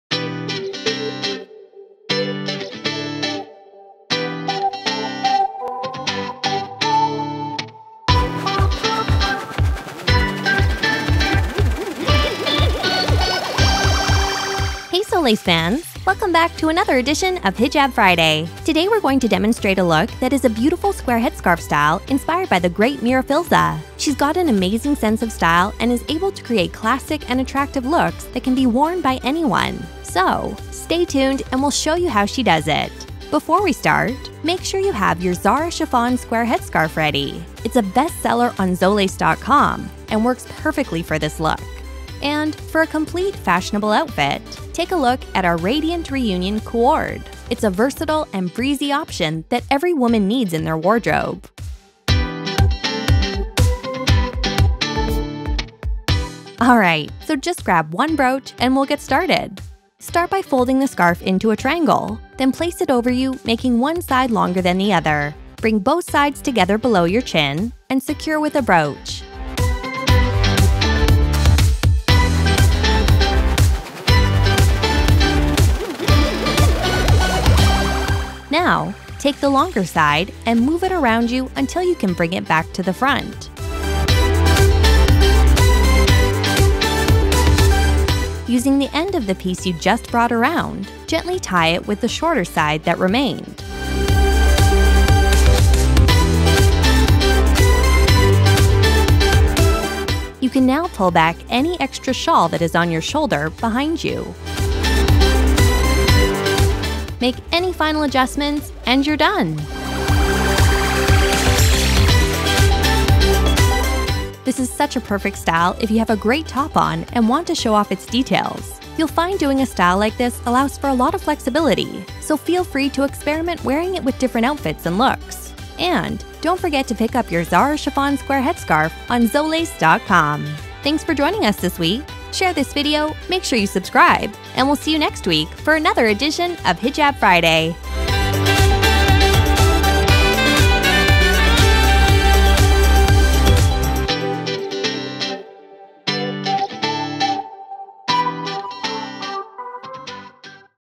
Hijab Tutorial 2017 - How To Rock A Classic Square Headscarf Style Inspired by Mira Filzah
Video Transcription

Today we’re going to demonstrate a look that is a beautiful headscarf style inspired by Mira Filzah. She’s got an amazing sense of style and is able to create classic and attractive looks that can be worn by anyone. Before we start, make sure you have your Zahra Chiffon Square Headscarf ready. It’s a best seller on Zolace.com, and work perfectly for this look.

To complete this look, we’ve paired it with the Radiant Reunion Co-Ord and Zahra Chiffon Square Headscarf

So just grab 1 brooch and we will get started!

Step 1 :Placing the shawl making one side longer than the other

Start by folding the scarf into a triangle, then place it over you making one side longer than the other.

Step 2 :Bring both sides together below your chin

Bring both sides together below your chin, and secure it with a brooch.

Step 3 :Take the longer side and move it around you

Now, take the longer side and move it around you until you can bring it to the front.

Step 4:Gently tie it with the shorter side

Using the end of the piece you just brought around, gently tie it with the shorter side that remained.

Step 5: Drape it over your head

You can now pull back any extra shawl that is on your shoulder behind you.

Final Look:

Make any final adjustment and you are done! This is such a perfect style if you have a great top on and want to show off its details. You’llÂ find doing a style like this allows for a lot of flexibility, so feel free to experiment wearing it with different outfits and looks. And, not to forget to pick up your Zahra Chiffon Square Headscarf on Zolace.com!

Hijab Friday Release Schedule:
We release our weekly Hijab Friday on Friday GMT+8 12PM.